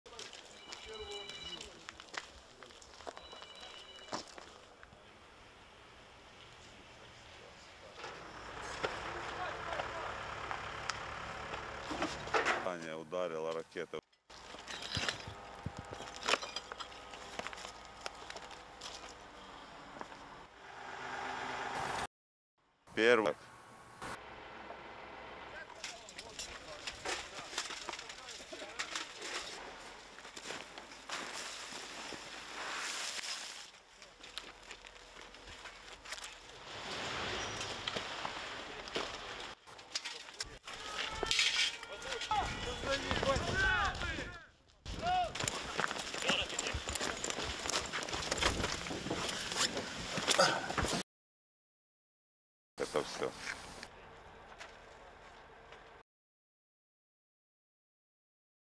Askeri Kışlanın Enkazında Canlı Bulma Umudu
Ukrayna’da  askeri kışlanın enkazında arama-kurtarma çalışmaları devam ediyor. Rusya, Mykolaiv kentindeki kışlayı Cuma günü hedef almıştı. Yetkililer saldırıda 40 askerin hayatını kaybettiğini belirtiyor